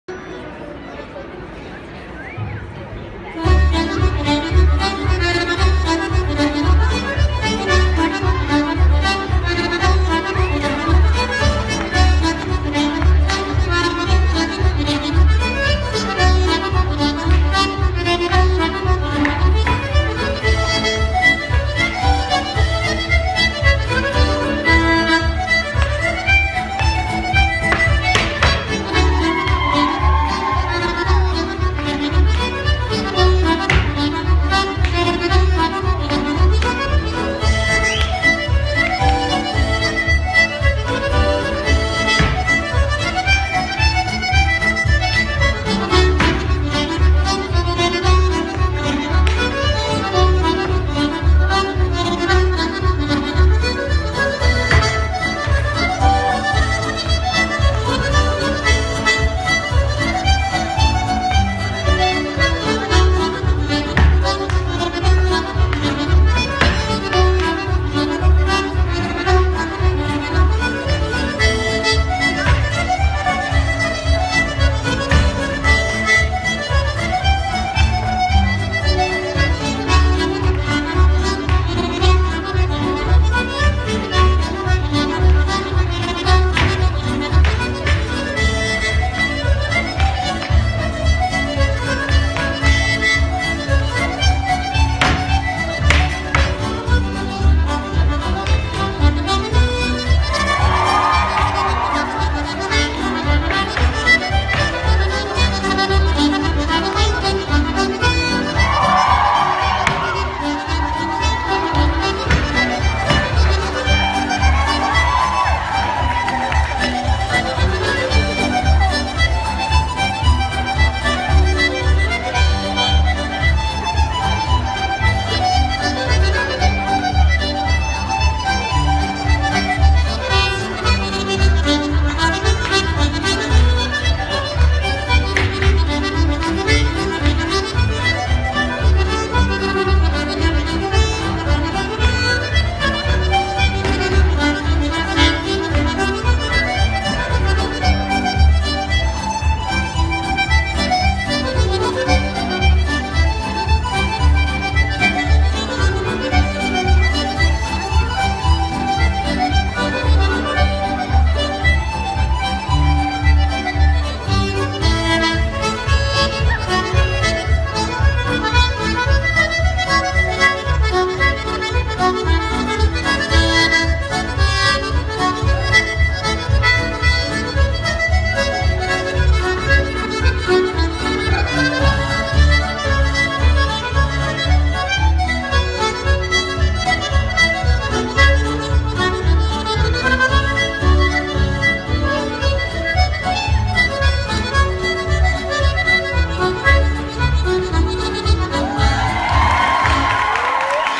Senior Reels - City of Birmingham Championship
Video credit to Alexander Kukharenko
John Whitehurst - Carey
Eamon Kitching - Conway Lally
Niall Forrest - McGahan Lees
Cian Walsh - Kiely-Walsh
Josh Ruddock - Ceim Oir
Scott Corden - Joyce-O'Donnell
Rohan Boyle - Caroline Greene
Jordan McCormick - Nolan-Bailey
Nina Rawlings - Doherty
Lauren Finglas - Carson Kennedy
Beth Monaghan - Carey